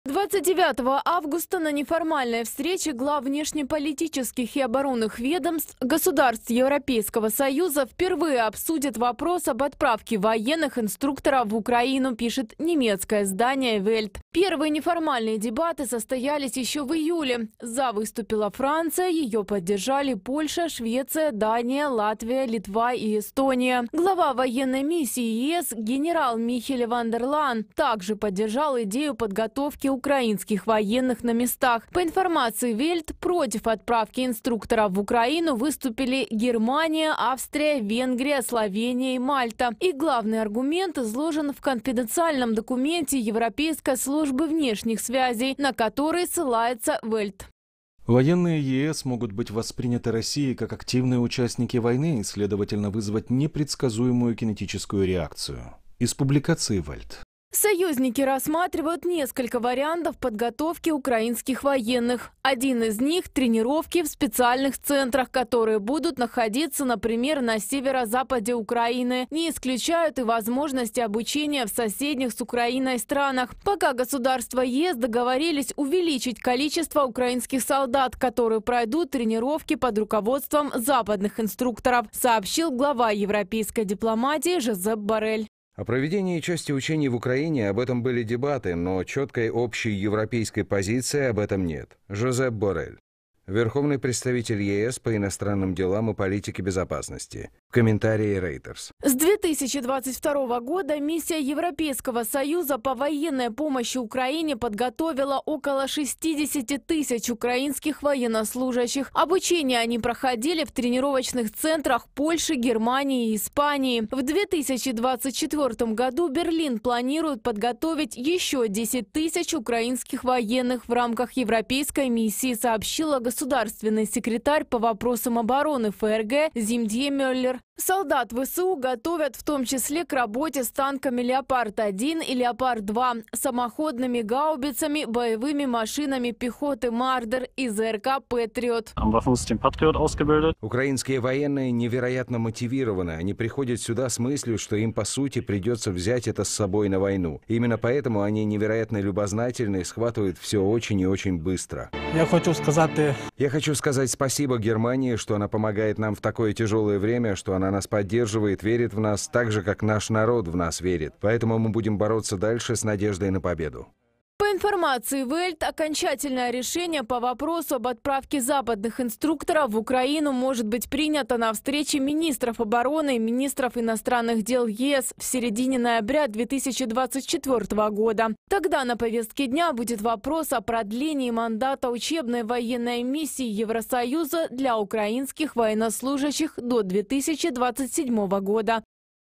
Если учится, то у лучших! ЕС отправит инструкторов ДЛЯ ВСУ! Безопасна ли такая помощь?!
В ЕС взвешивают риски отправки военных в Украину. Страны Европы явно заинтересованны в решении этого вопроса. ЕС начинает разворачивать такой внутренний конфликт, которого не было раньше. Так стоит ли ожидать помощи от ЕС в ближайшее время? Все детали смотрите в нашем видео!